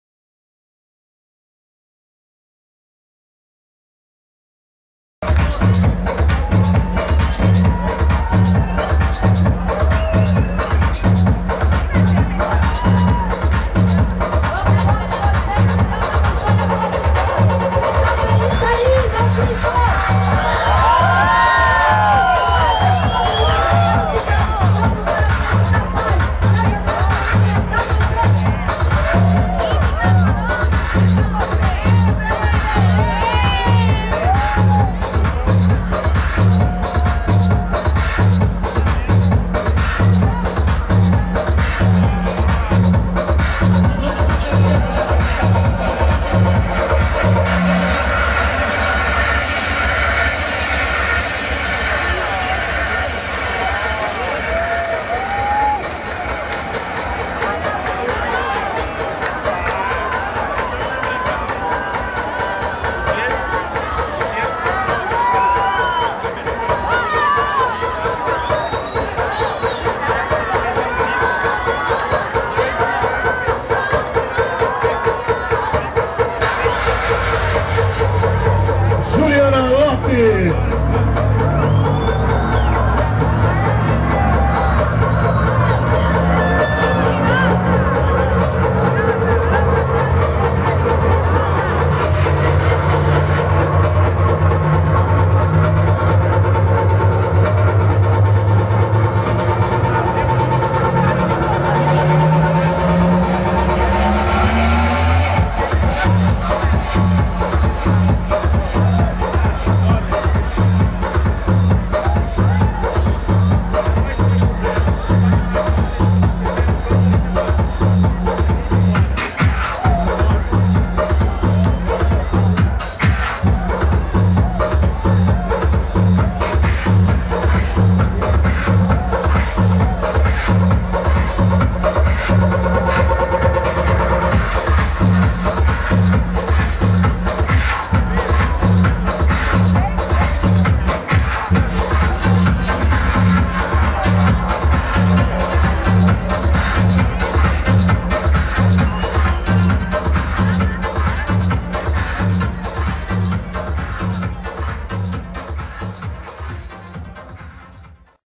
DJ Juliana Lopes na Virada Cultural SP 2010
Juliana Lopes no Largo do Arouche, São Paulo,  Virada Cultural 2010